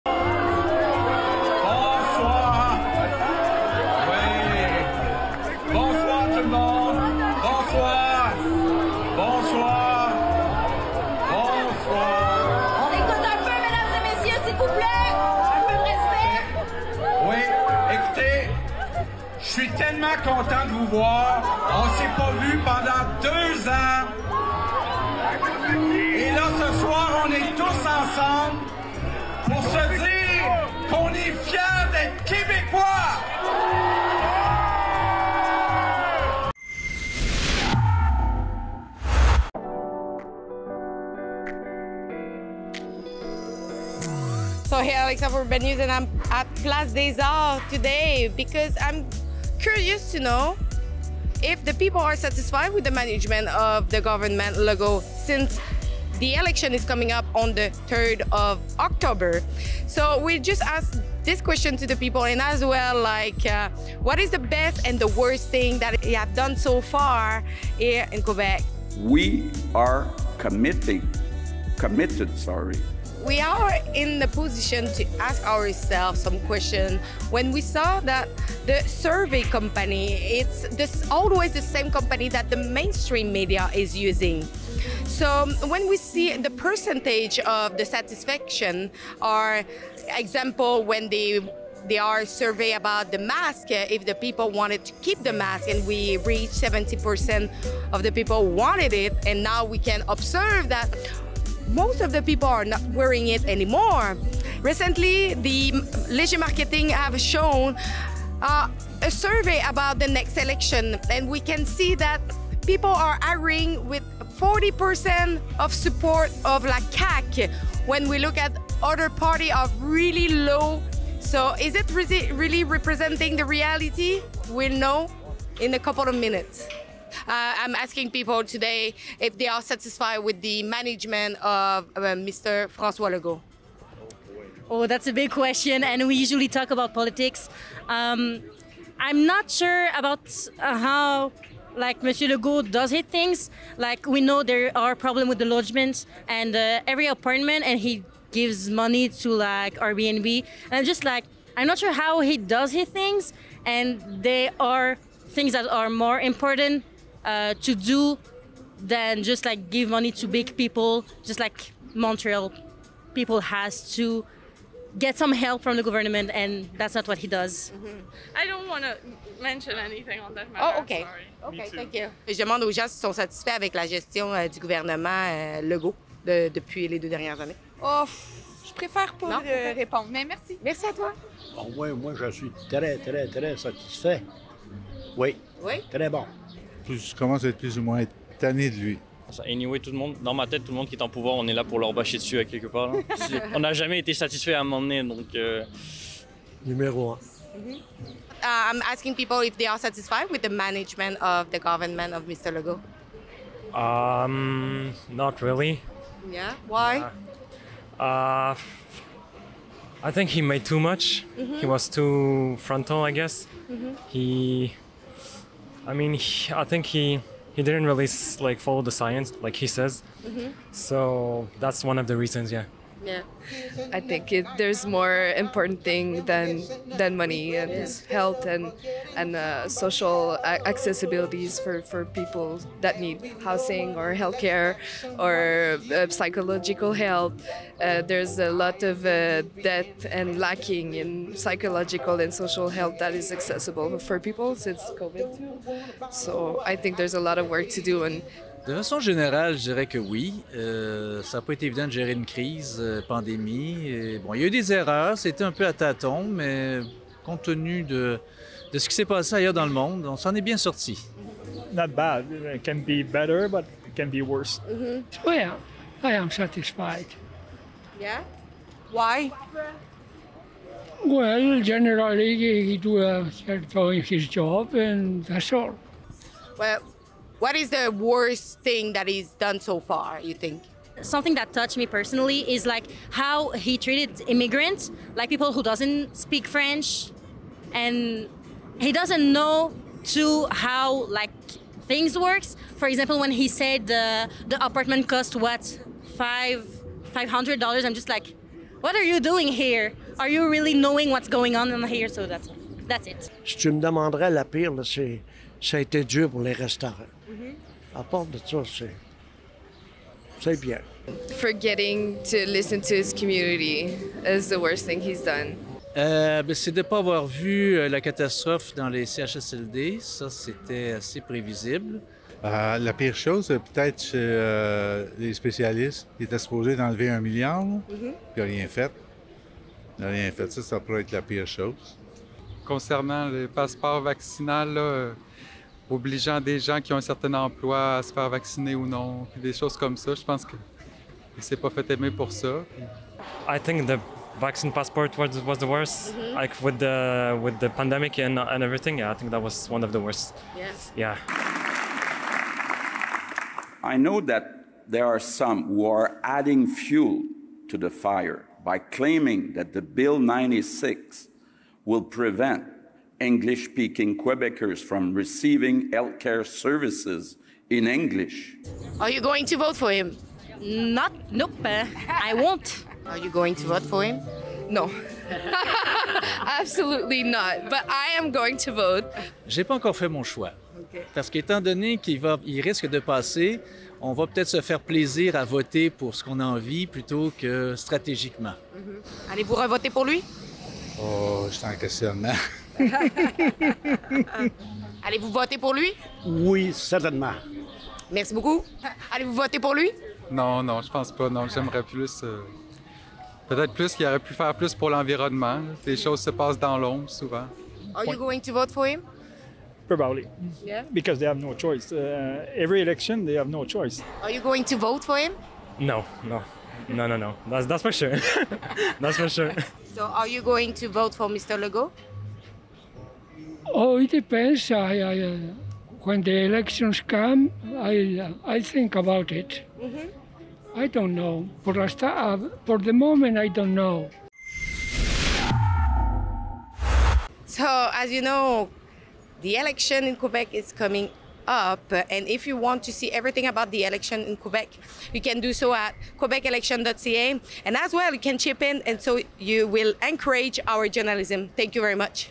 Are Quebecers satisfied with the leadership of Premier Legault?
We asked Montreal residents if they are satisfied with the leadership of Mr. François Legault within the CAQ party.